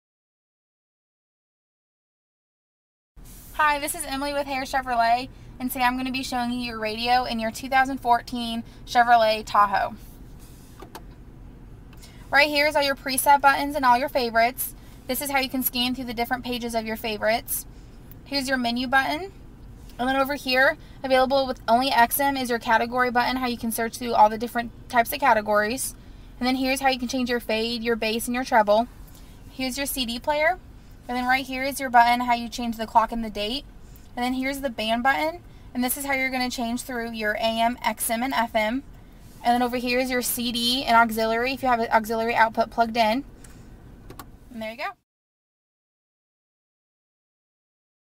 How To: Program Your Radio in the 2014 Chevrolet Tahoe Indianapolis
Emily Robison from Hare Chevrolet explains how to program the radio in the 2014 Chevrolet Tahoe.